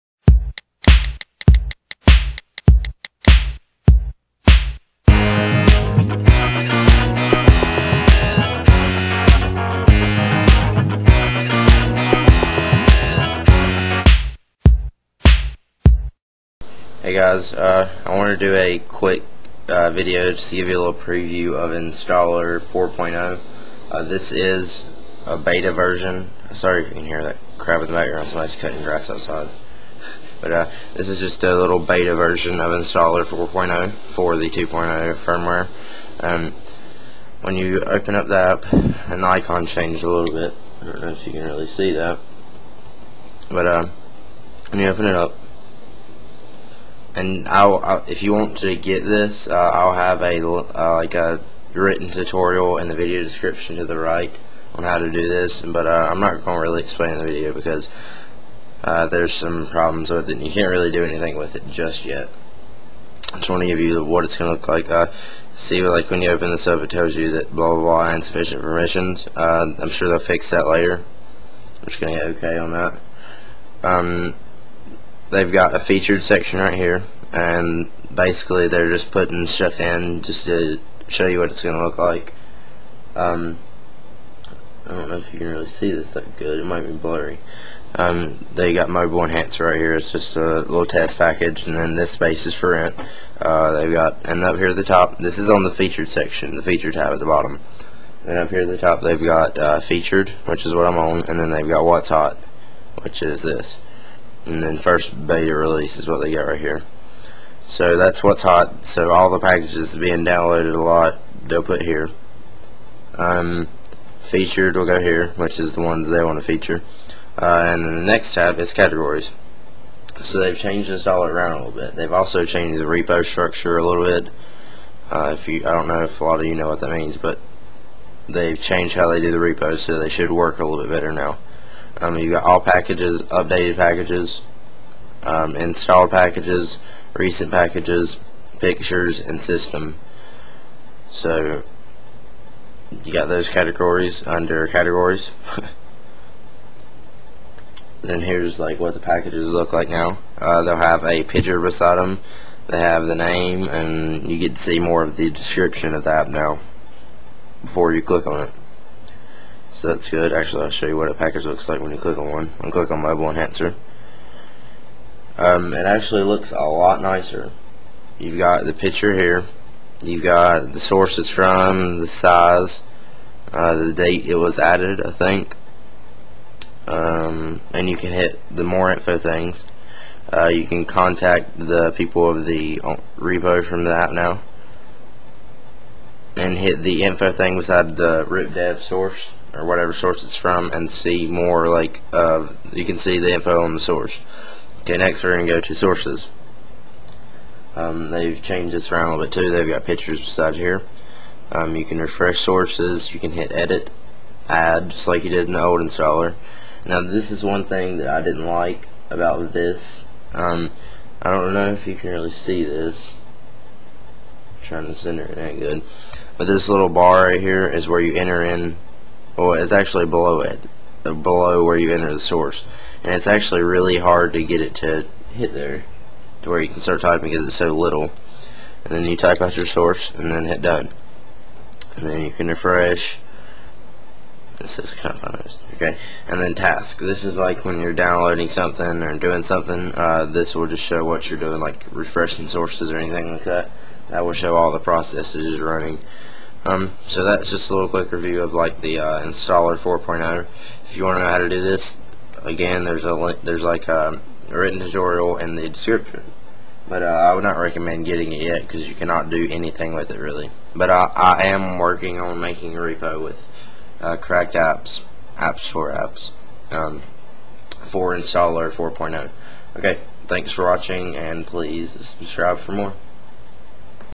Installer 4.0 Beta Preview
I do not recommend installing this right now seeing as how it is a beta version and you can't do much with it right now. But as soon as the final version is released i will make a video to inform you guys.

To Install:
1. download installer from the link above and save it to the desktop
2. ssh
3. unzip installer folder
4. in winscp(or whatever your using) navigate to private/var/mobile/stash
5. drag the installer.app folder into the applications.sdkgjs(extension varies)
6. go into the applications.sldks folder and change permissions of the installer.app folder to 775 and select the box beside "set group owner and permissions recursively"
7. reboot your iphone/ipod